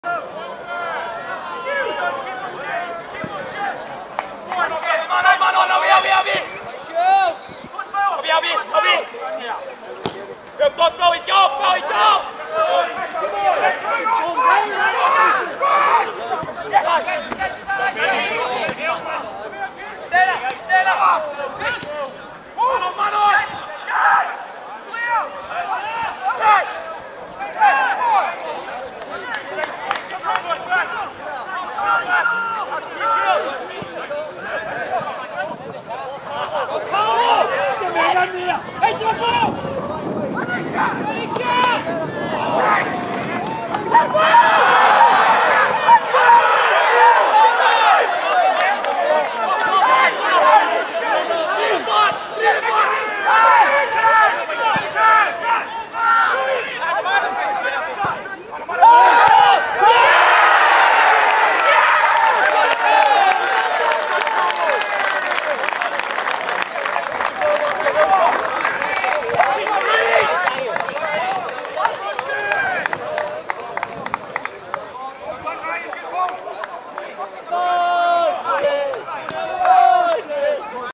carlo monti 2v0 41m cambuslang 0v6 pollok 22.9.2102
carlos 2nd goal from the scottish cup tie against cambuslang at somervell park